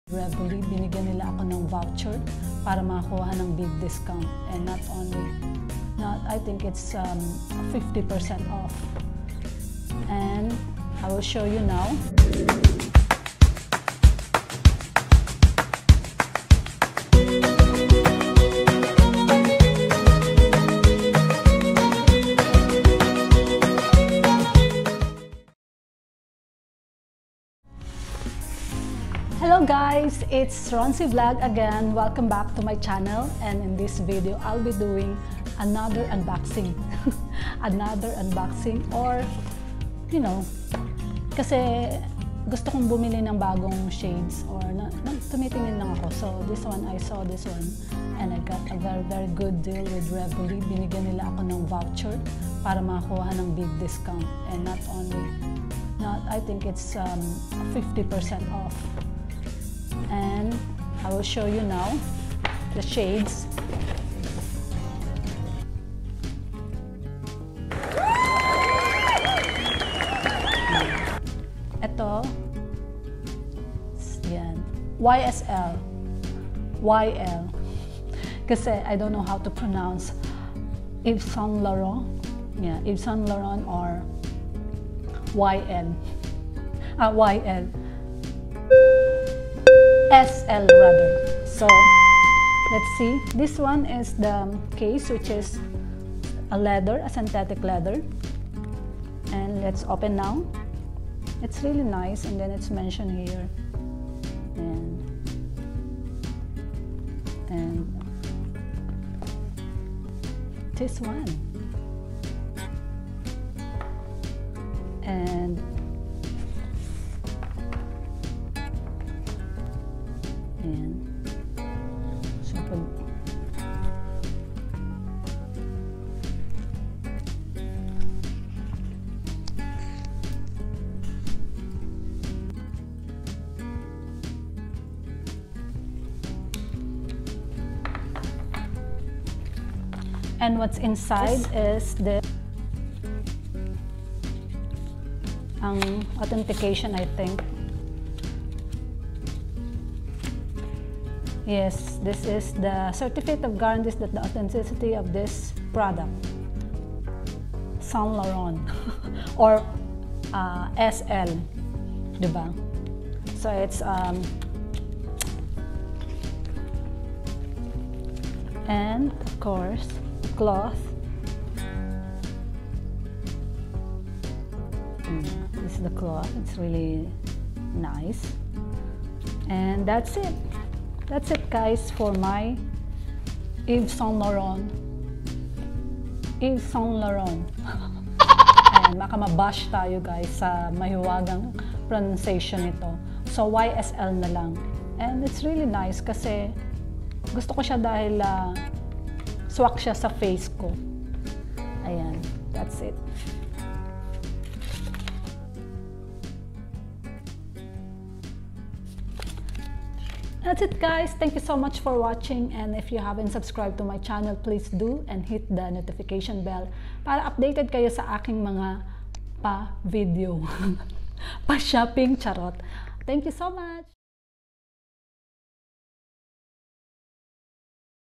Saint Laurent Sunglasses Unboxing and Review
Another unboxing and review of my Saint Laurent Sunglasses SL 406 from Rivoli Eye Zone .

@The Rhonzie Vlog